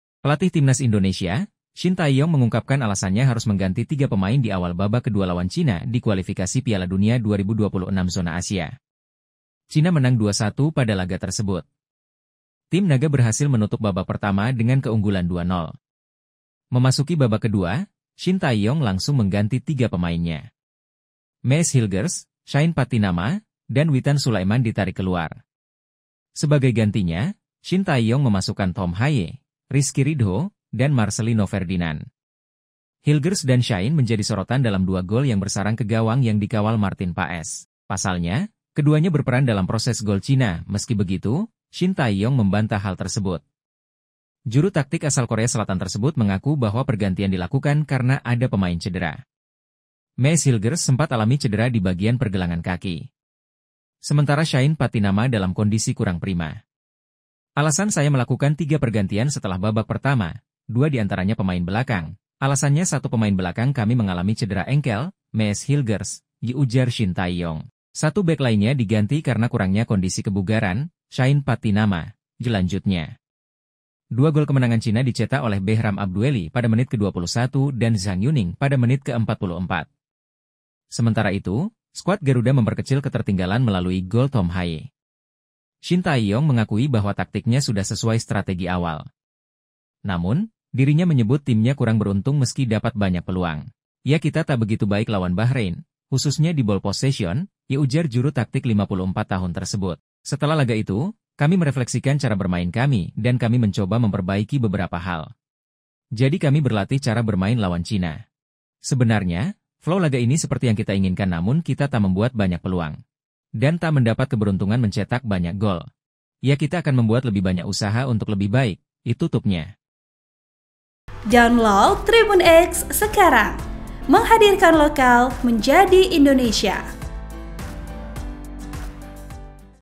Ini Alasan Shin Tae-yong Lakukan 3 Pergantian Pemain Pada Awal Babak Kedua Lawan China
Pelatih Timnas Indonesia, Shin Tae-yong mengungkapkan alasannya harus mengganti tiga pemain di awal babak kedua lawan China di Kualifikasi Piala Dunia 2026 Zona Asia. China menang 2-1 pada laga tersebut. Tim Naga berhasil menutup babak pertama dengan keunggulan 2-0.
Memasuki babak kedua, Shin Tae-yong langsung mengganti tiga pemainnya. Mees Hilgers, Shayne Pattynama, dan Witan Sulaeman ditarik keluar.
Sebagai gantinya, Shin Tae-yong memasukkan Thom Haye, Rizky Ridho, dan Marselino Ferdinan.
Hilgers dan Shayne menjadi sorotan dalam dua gol yang bersarang ke gawang yang dikawal Maarten Paes. Pasalnya, keduanya berperan dalam proses gol China. Meski begitu, Shin Tae-yong membantah hal tersebut. Juru taktik asal Korea Selatan tersebut mengaku bahwa pergantian dilakukan karena ada pemain cedera. Mees Hilgers sempat alami cedera di bagian pergelangan kaki. Sementara Shayne Pattynama dalam kondisi kurang prima.

Editor Video : Petrus Lewo Hekin (PH)